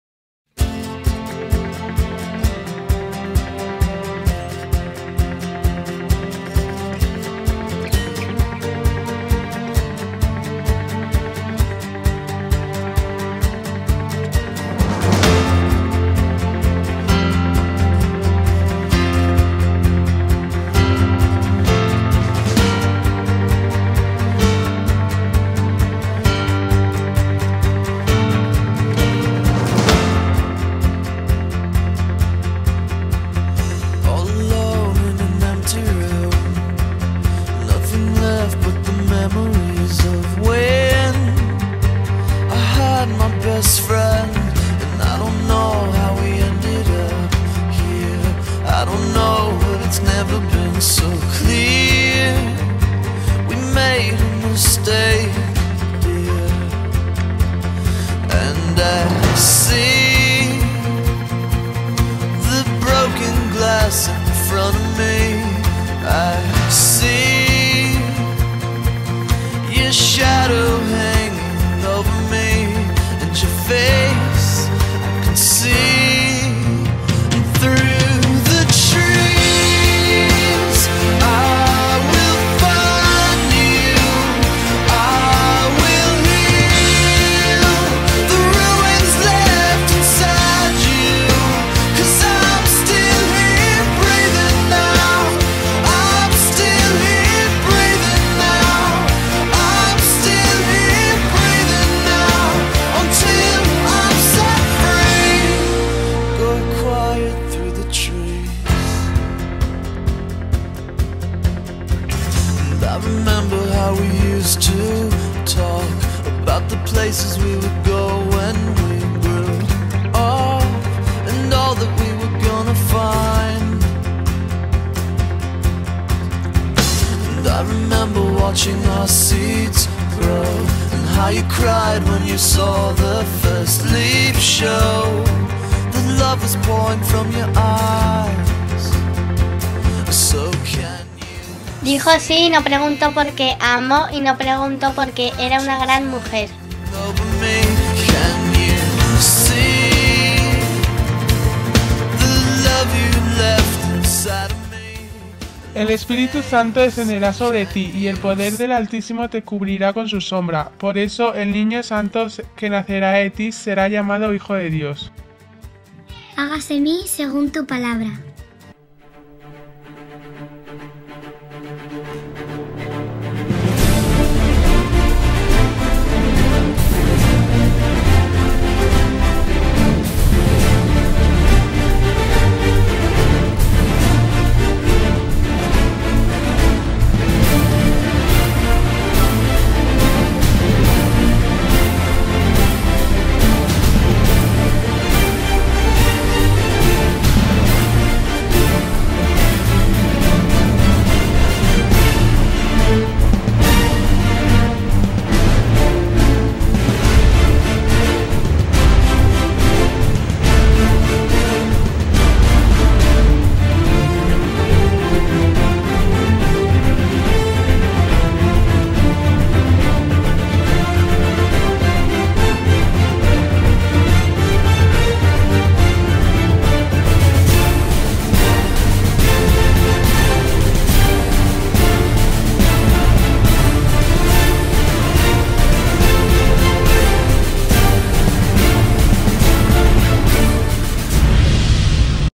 2º Premio "Carii Fillii" Alumnas Everest Monteclaro.m4v
Vídeo ganador del 2º premio en el concurso Carii Filli, por nuestras alumnas del Colegio Everest Monteclaro. ¡Enhorabuena!
Proud to be Everest!